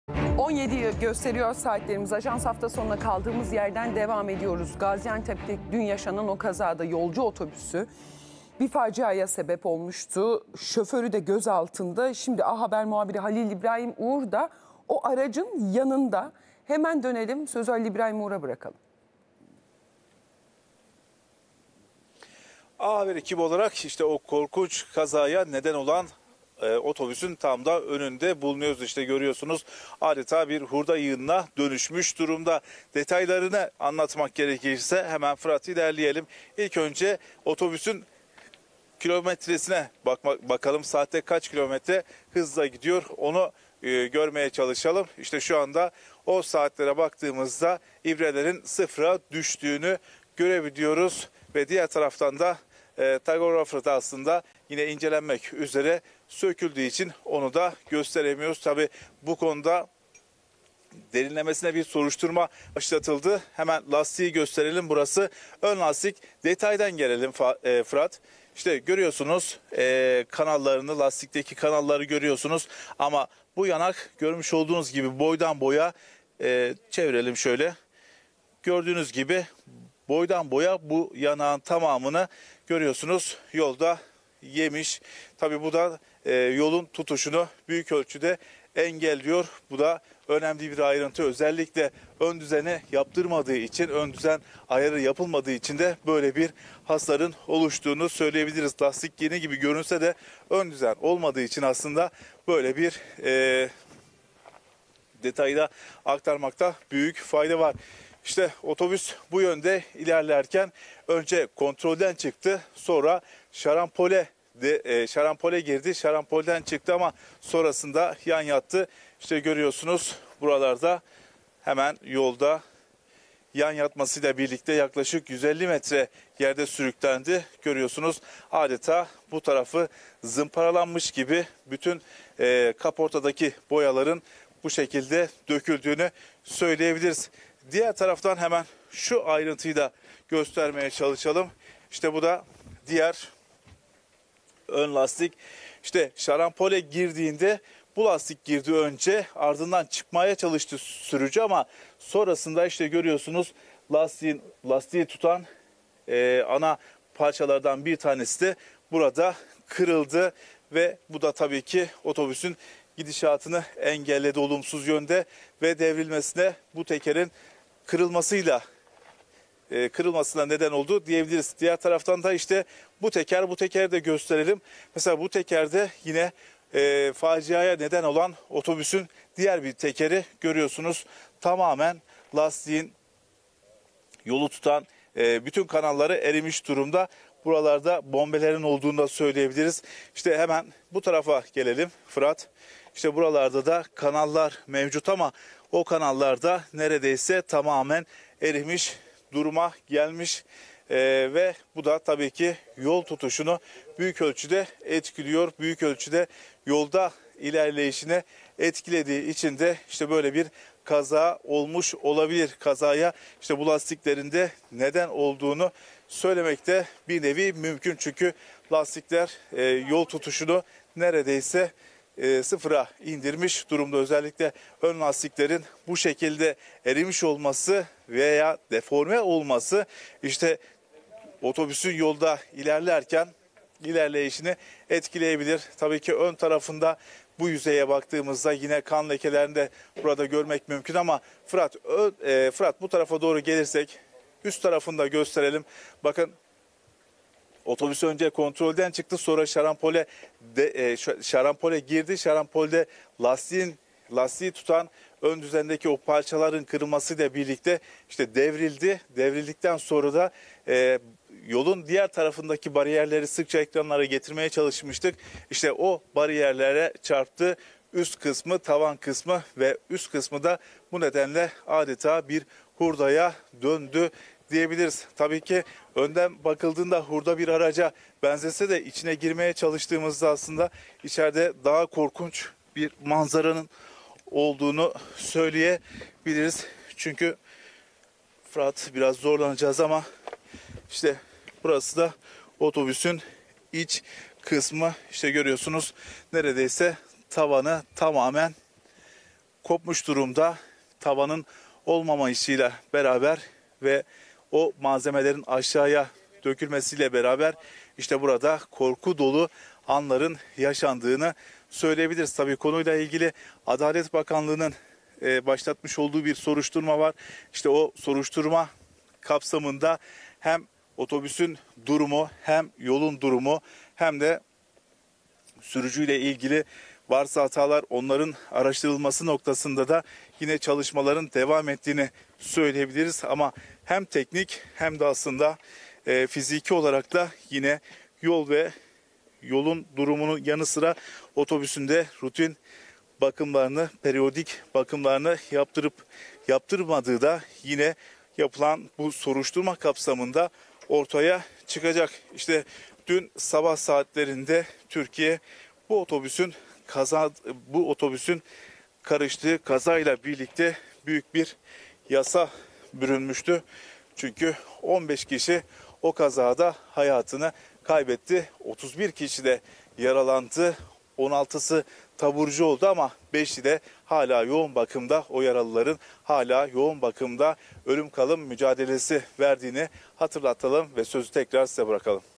Gaziantep'teki kazada 15 kişi ölmüş, 21 kişi de yaralanmıştı! işte faciaya neden olan otobüs!
Gaziantep'te katliam gibi bir kaza meydana geldi. Yolcu otobüsü, ambulans, itfaiye aracı ve gazetecilerin canlı yayın aracının karıştığı kazada 15 kişi hayatını kaybetmiş, 21 kişide yaralanmıştı. İşte faciaya neden olan otobüsten kareler...